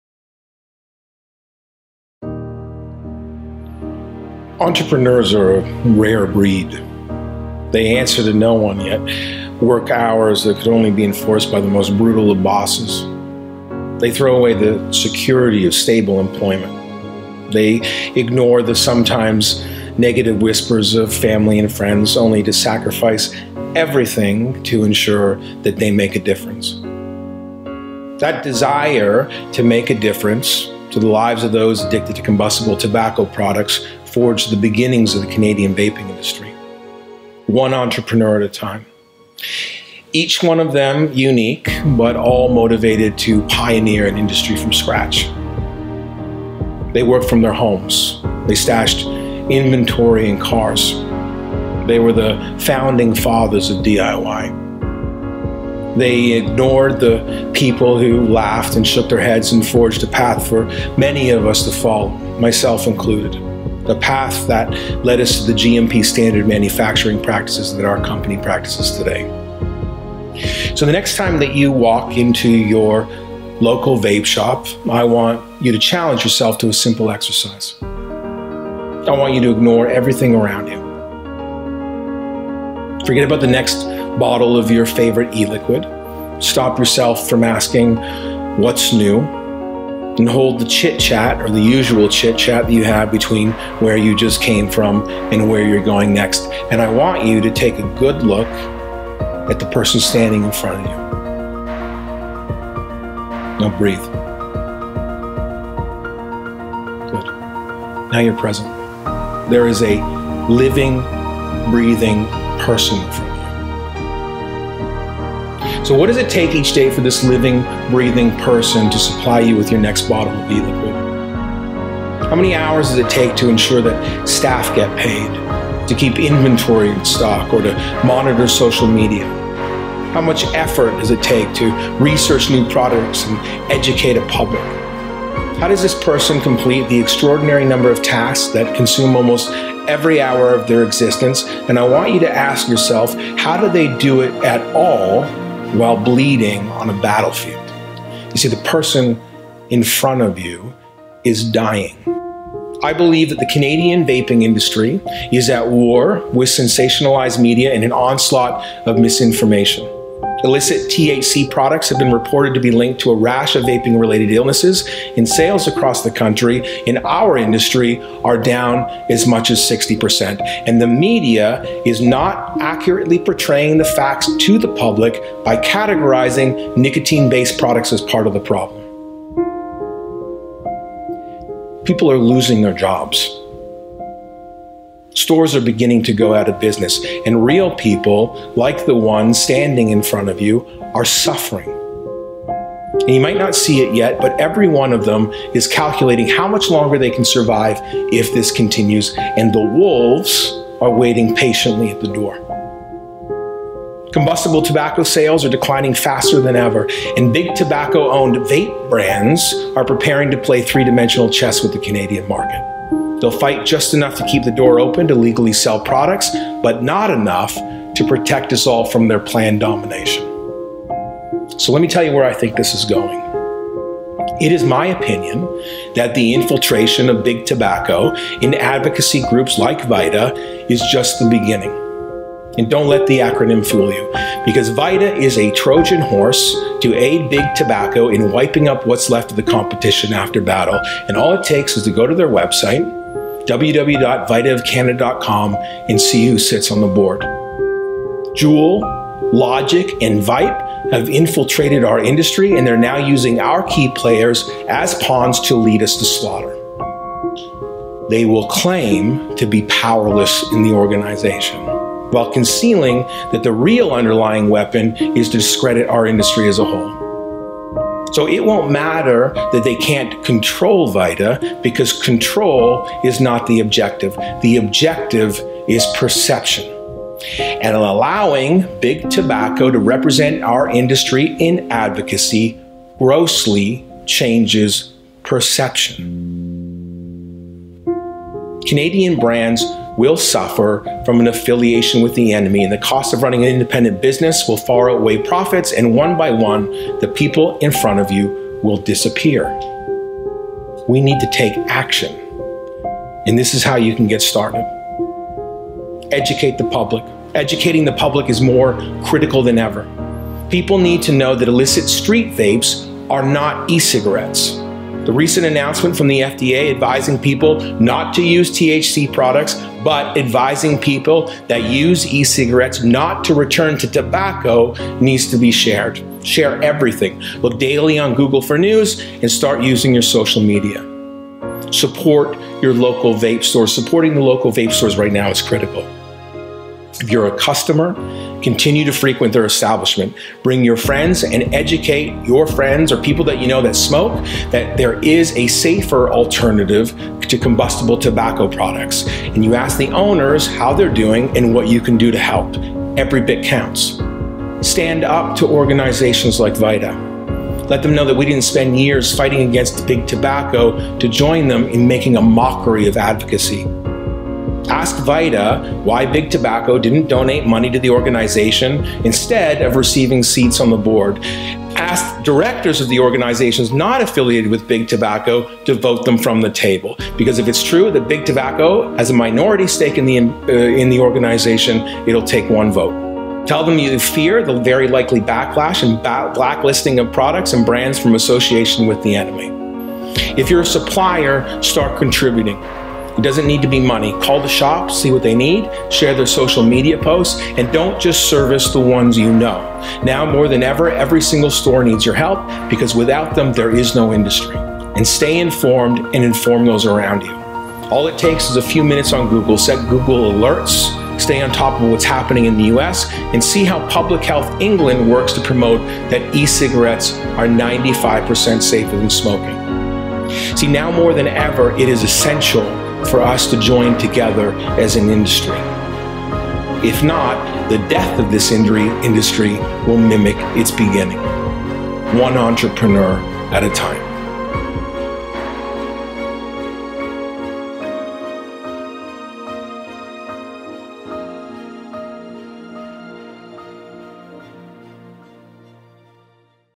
We Are Not Big Tobacco - A message from the Vape Industry
A special edit I did of David Richmond-Peck's video from a few weeks ago. THIS IS POWERFUL! Please spread and share this with everyone you know!